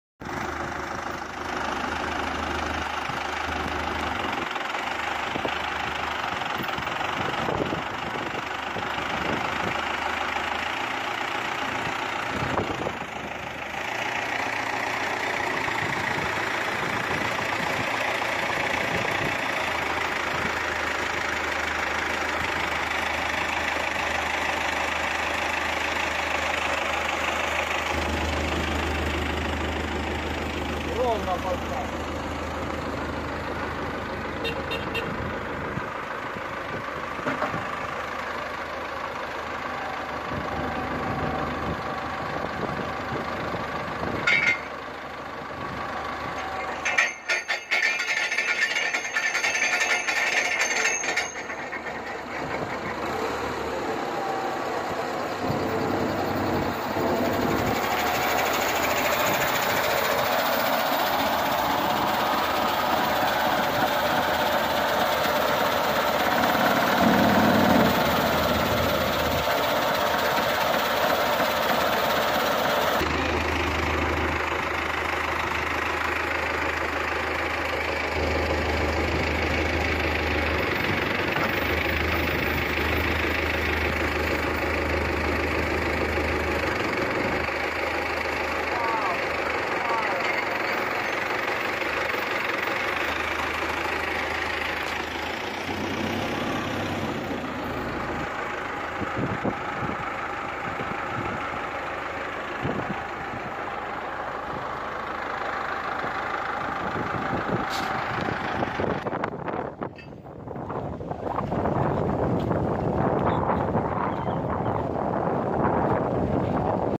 косилка - мульчер DOBERKMET AGF200
Косилка-измельчитель для ухода за садами, парками, обочинами дорог и борьбы с сорняками